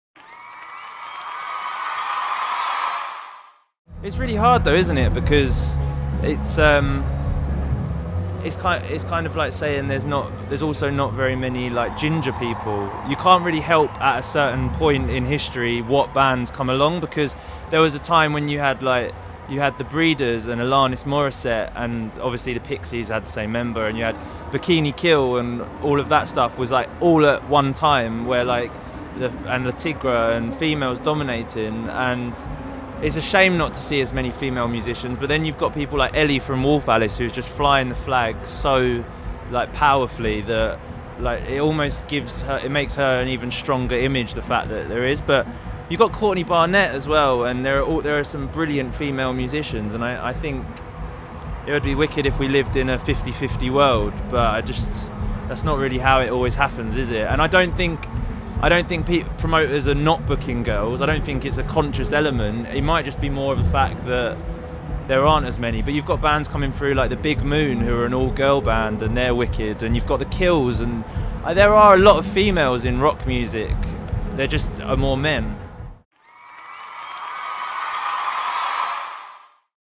Reading Festival 2016: Slaves on the lack of women in rock music
Slaves on the lack of women in rock music